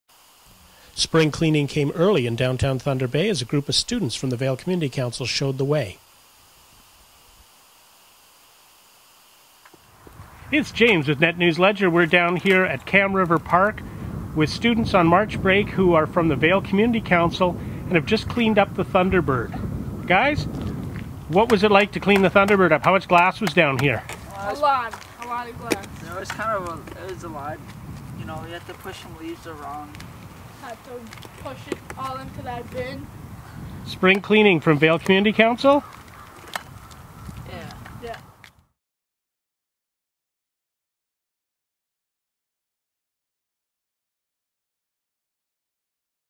Vale Community Council Steps Up to Clean Up
THUNDER BAY - NEWS - Spring Cleaning got an early start today as youth from the Vale Community Council stepped up to clean up the Thunderbird at the Kam River Park in downtown Fort William.

Over the winter a great deal of broken glass at the entrance to the park has gathered up.

Thunder Bay Parks do not, as a practice maintain walking trails at many parks. Over the winter broken glass needles, and other litter along with solid evidence of a fair amount of vandalism has been accumulating at Kam River Park.

Thunder Bay City Manager Norm Gale offers, "Regarding maintenance at this park I offer the following comments our parks dept:

"Parks has a different standard for level of service than Roads sidewalks do. Parks trails that are plowed are not sanded. And some trails we make a decision not to plow, for example, the Darrell Matson trail along Golf Links is not plowed. In general we try to stay away from sanding, even when icy conditions are found as it sets an expectation that we cannot maintain.

"Having said that, we are initiating a survey for residents to comment on what changes they would like for input into the Roads Quality Standards report and have included questions on the service levels for multi-use trails and bike lanes. So we will have some data soon on what residents would like to see.

"I am mindful of the importance of quality open and green spaces. As you can see above, we are seeking public input into the standards for this".

Mayor Hobbs states, "This park is never maintained properly. Tim Commisso, Darrell Matson and I have all walked it and I ‎have been on Administration's case about maintaining it."

Thunder Bay has a goal of building up the city's waterfront. The Kam River Park has the historic James Whalen Tugboat and the VIA Rail train, as well as a scenic boardwalk. With proper maintenance, the park would like attract a lot more people to enjoy the vista and views.